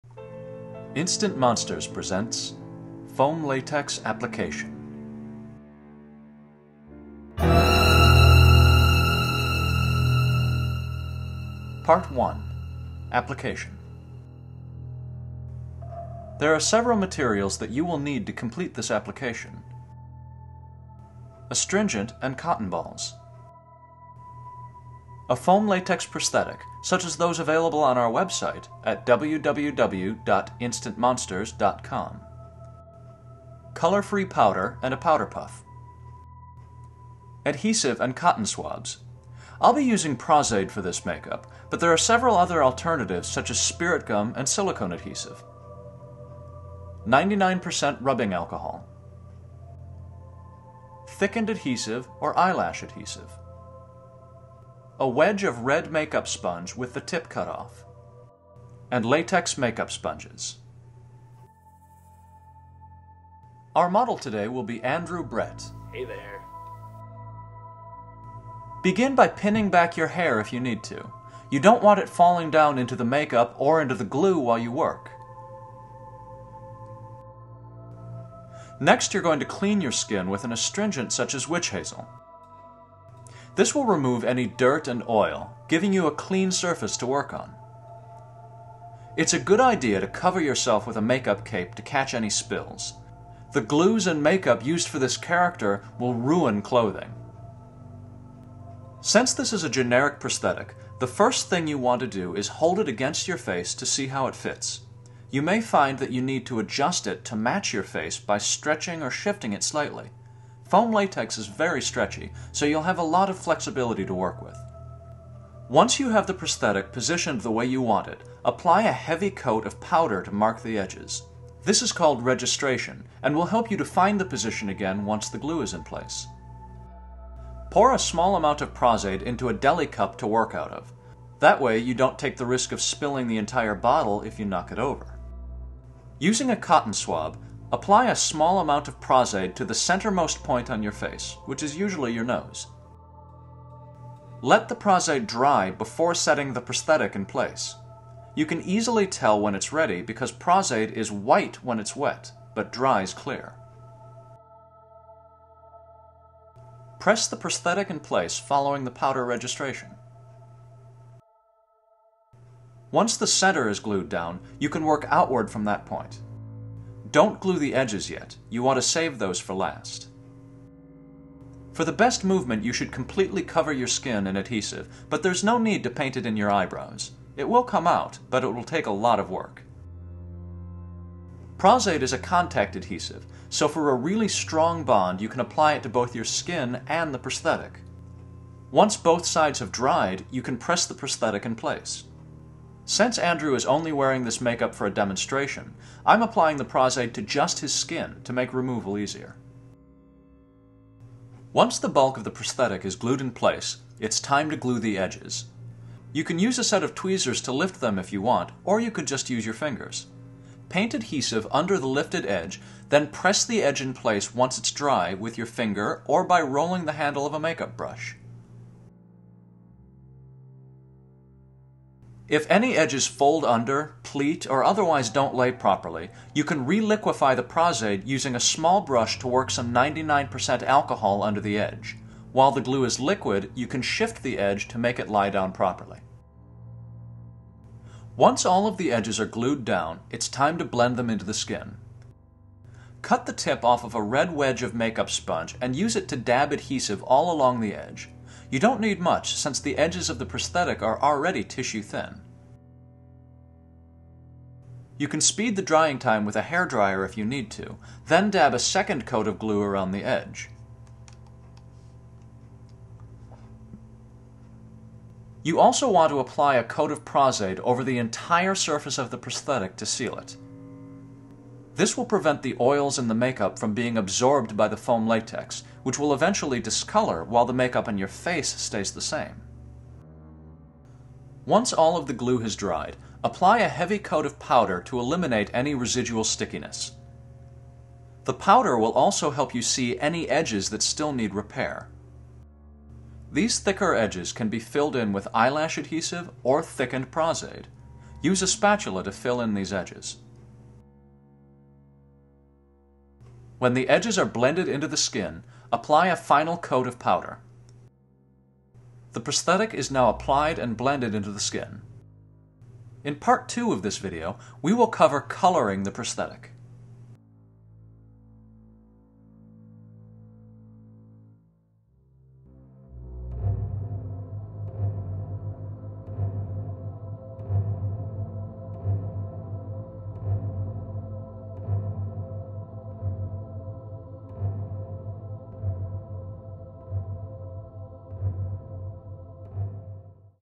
OLD Foam Latex Makeup Application - part 1
Learn how to apply, color and remove foam latex prosthetics with this tutorial from Instant Monsters.